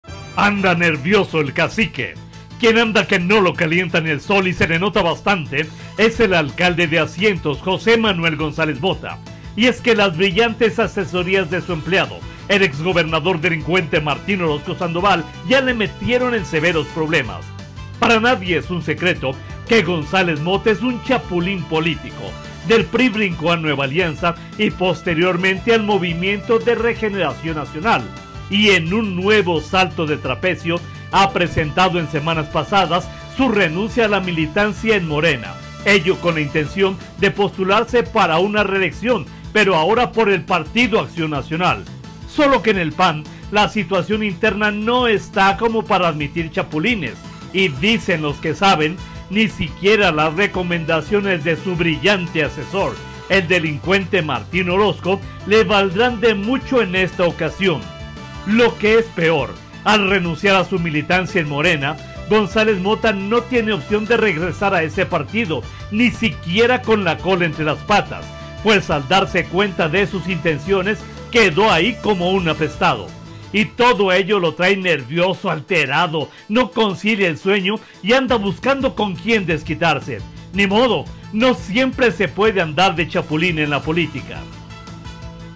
Crónica del Poder/Anda nervioso el cacique
Anda nervioso el cacique

Aguascalientes, Ags. 22 mayo 2023.- Quien anda que no lo calienta ni el sol, y se le nota, es el alcalde de Asientos, José Manuel González Mota. Y es que las brillantes asesorías de su empleado, el exgobernador delincuente Martín Orozco Sandoval le han metido en severos problemas.
Para nadie es un secreto que González Mota es un chapulín político. Del PRI brincó a Nueva Alianza y posteriormente al Movimiento de Regeneración Nacional y, en un nuevo salto de trapecio, ha presentado ya su renuncia a la militancia en Morena, con la intención de postularse para una reelección por el Partido Acción Nacional.
Sólo que en el PAN la situación interna no está como para admitir chapulines externos y, dicen los que saben, ni siquiera las recomendaciones de su “asesor”, el delincuente Martín Orozco Sandoval, le valdrán de mucho en esta ocasión.
Lo que es peor, al renunciar a su militancia, González Mota no tiene opción de regresar a Morena ni siquiera con la cola entre las patas, pues al darse cuenta de sus intenciones, quedó como apestado.
Y todo esto lo trae nervioso, alterado, no concilia el sueño y anda buscando con quien desquitarse. Ni modo, no siempre se puede andar de chapulín.